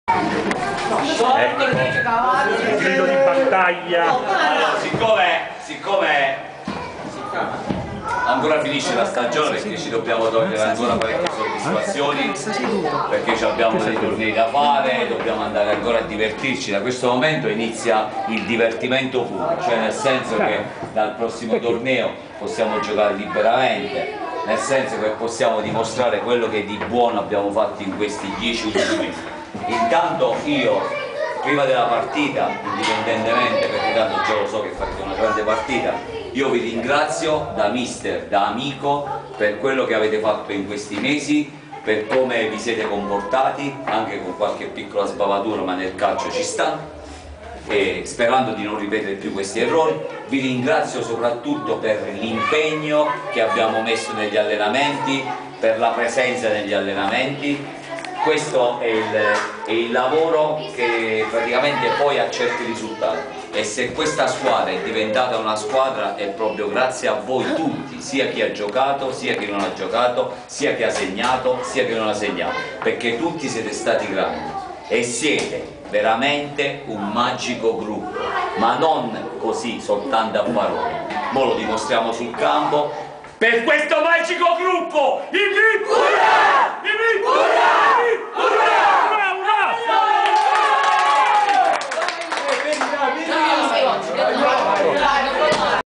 VIRTUS CASTEL FRENTANO " GIOVANISSIMI " Stag. 2016 - 2017
Gara VIRTUS CASTEL FRENTANO - MIGLIANICO CALCIO
Mancano una manciata di minuti all'inizio della Gara . Mister Gabriele tiene il suo " sermone "  preparando i suoi ragazzi  alla Gara .